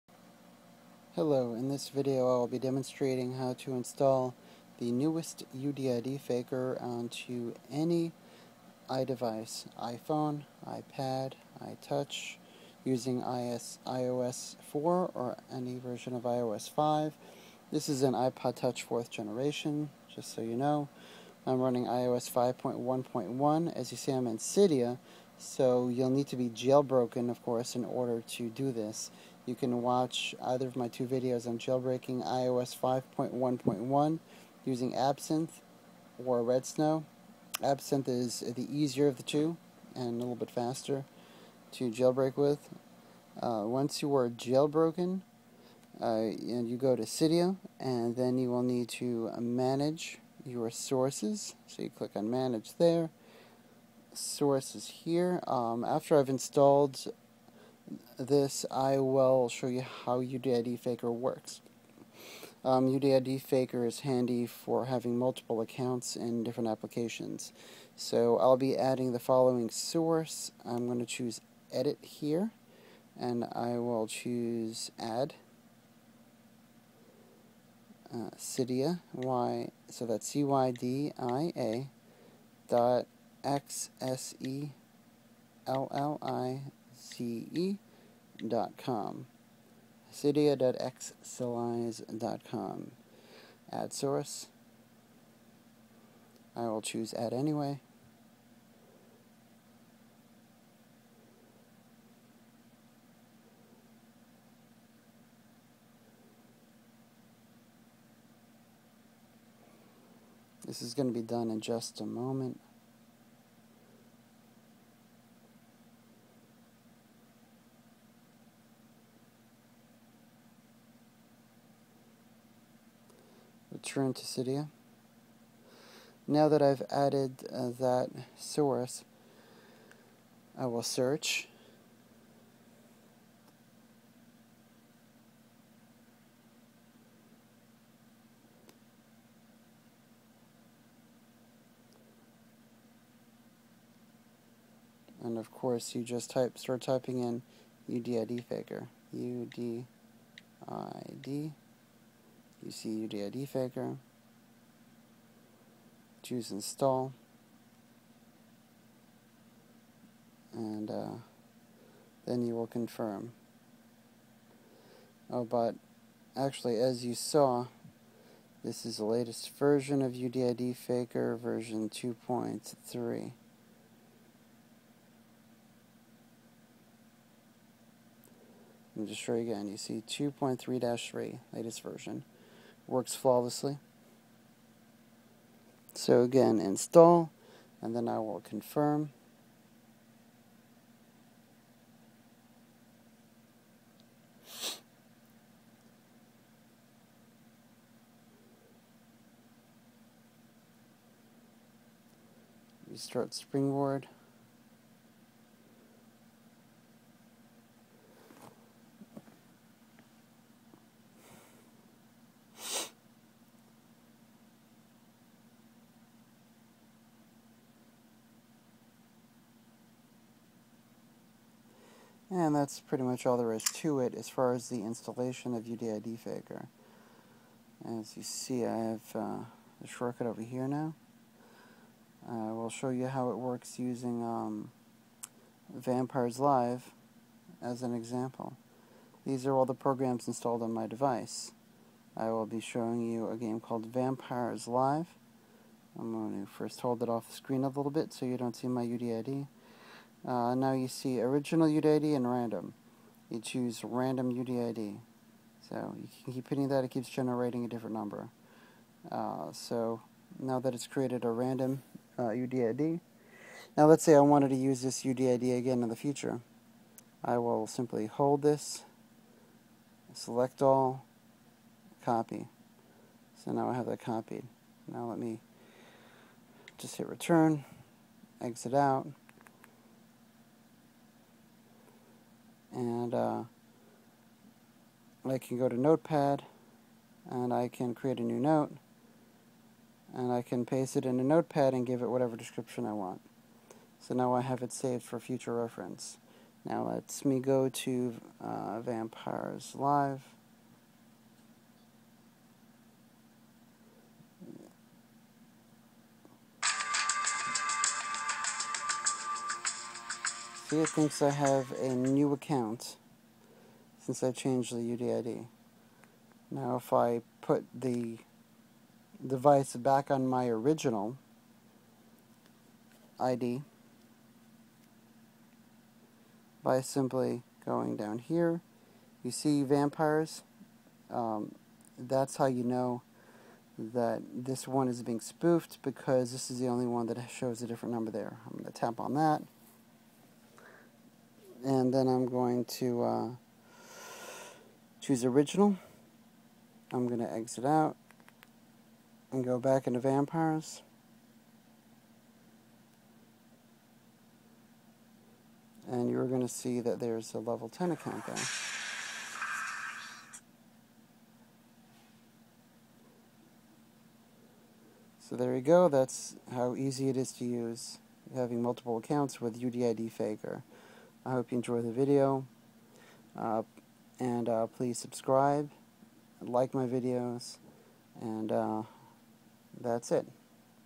How to Install the NEW UDID FAKER 2.3-3 & use multiple accounts on any iPhone iPad or iTouch
This is a video tutorial on how to install the latest version of UDID Faker 2.3-3 and a demonstration on how it works using a Storm8 Game as an example. This program is easy to install and use if you follow my video.  You must be jailbroken on any iOS 4 or 5 to install UDID Faker. You can watch one of my videos on Jailbreaking iOS 5.1.1 using either Absinthe or Redsn0w if you're not yet jailbroken and use an iOS 5.1.1 device.

P.S. UDID Faker does not currently work on iOS 7 and above.  It works on iOS 6 and below.